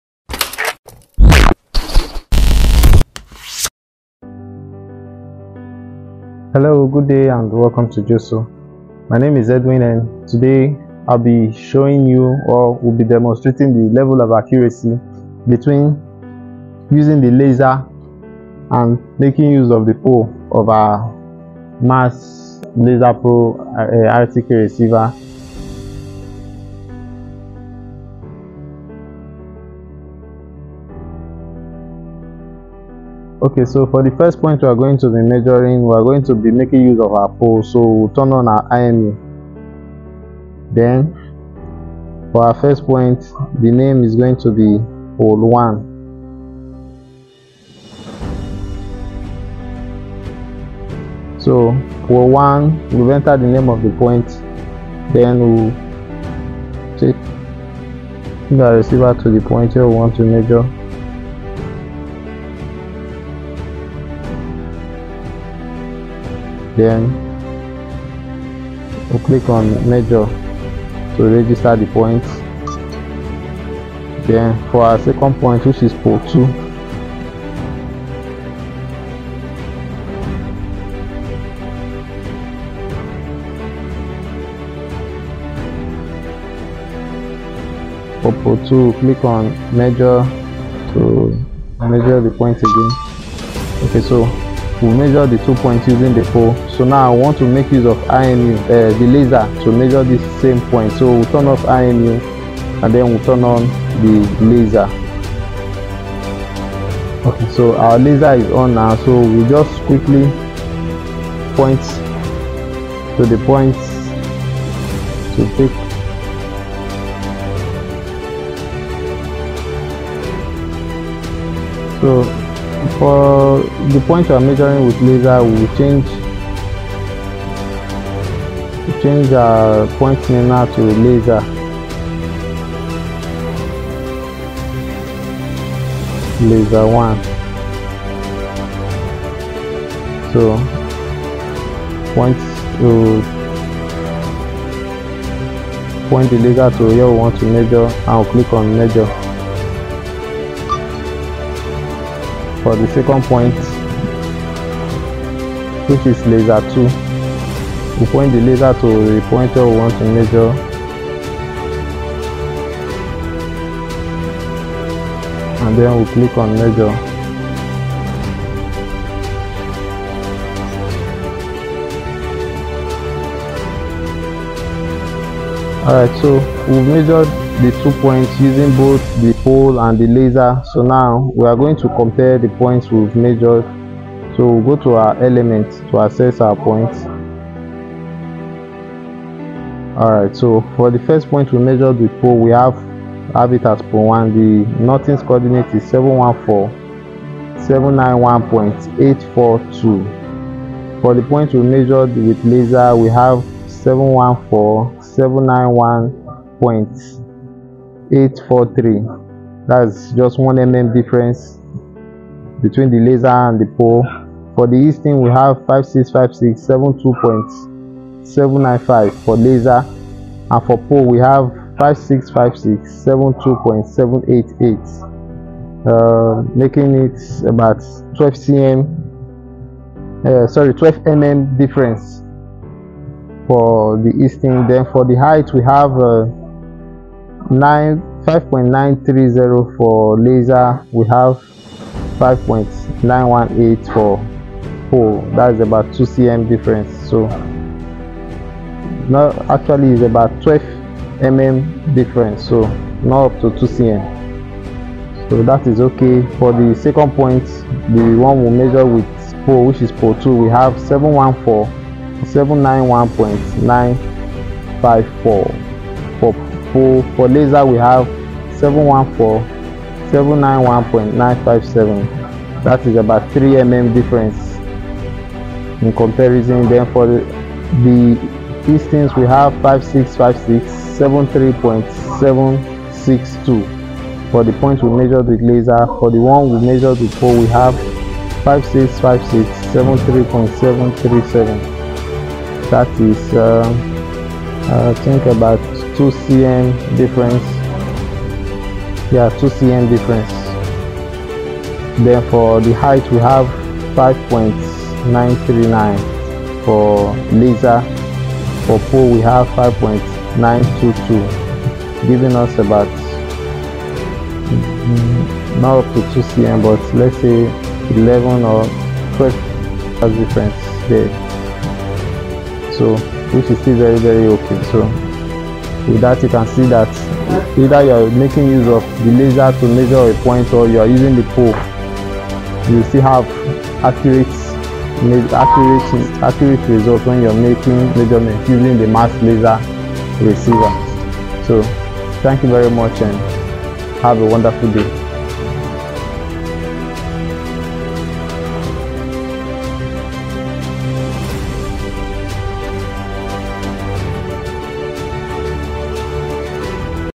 GNSS TUTORIAL - COMNAV SINOGNSS MARS PRO LASER RTK ACCURACY TEST
In this video, we compare the accuracy of the laser of the Mars Pro with the values obtained from using the conventional RTK pole